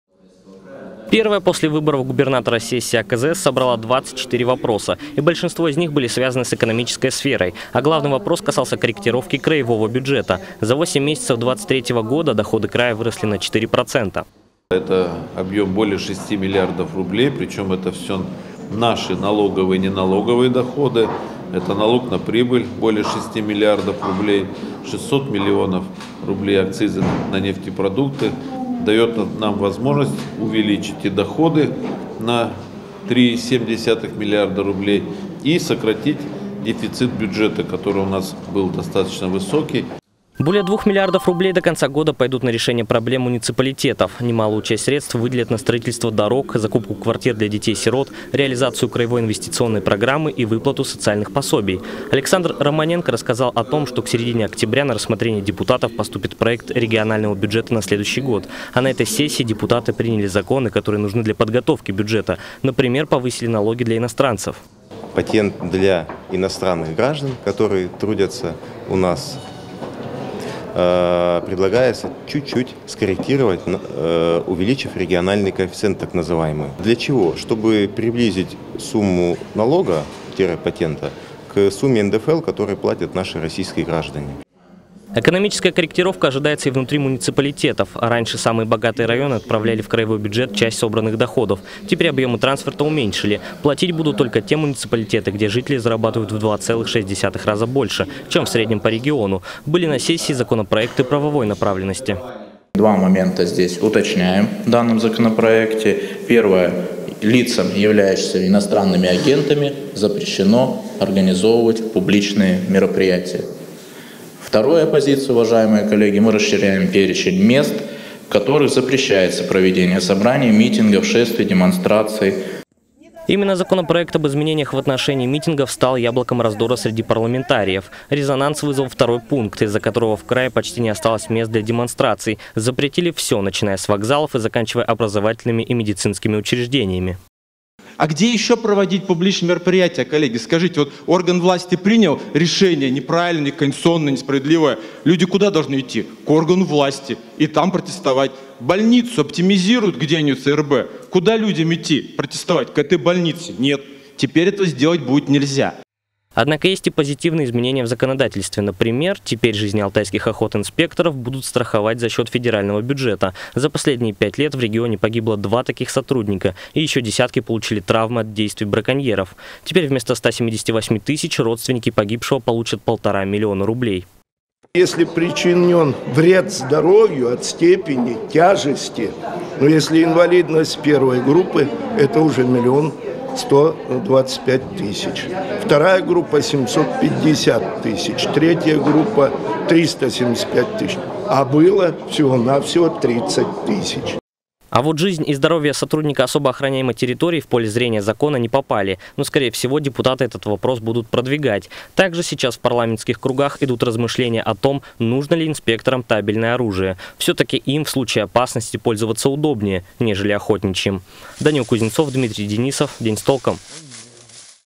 С новым депутатом и отредактированным бюджетом: как прошла сессия АКЗС
Новые запреты для митингующих, страхование жизни охотинспекторов и экономическое будущее Алтайского края. Такие темы были на повестке сентябрьской сессии законодательного собрания региона. Особенное внимание парламентариев привлекла корректировка краевого бюджета этого года.